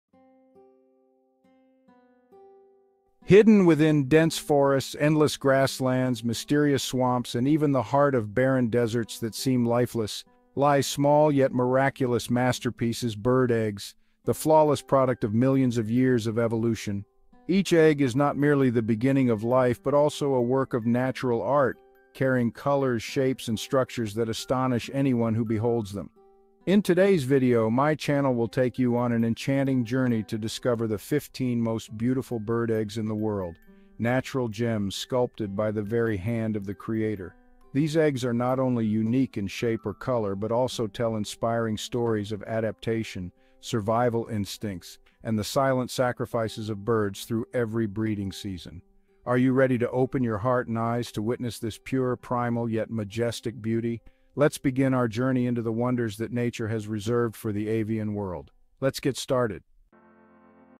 Top Most Fascinating Bird Eggs in the World – Nature’s Tiny Masterpieces
Discover the most fascinating and extraordinary bird eggs on the planet – from those laid on sheer cliff edges, uniquely shaped so they won’t roll off, to the giant deep-emerald eggs of the cassowary and the rare, fiery-red eggs of the Tsetse warbler. Each egg is a miniature masterpiece, meticulously crafted by nature to protect life. Join us in admiring their fragile beauty, incredible survival stories, and the evolutionary secrets hidden within every delicate shell.